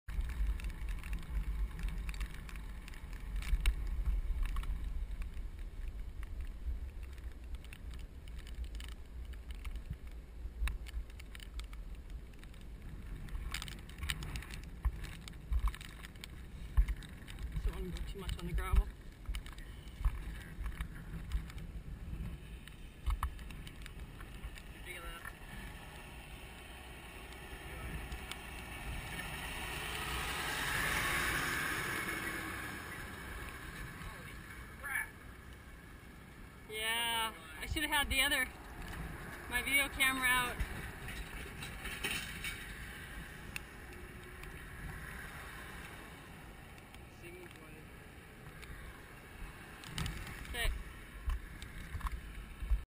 Windmill move Maui
Cycling in Maui - cool to see a part of a windmill being moved.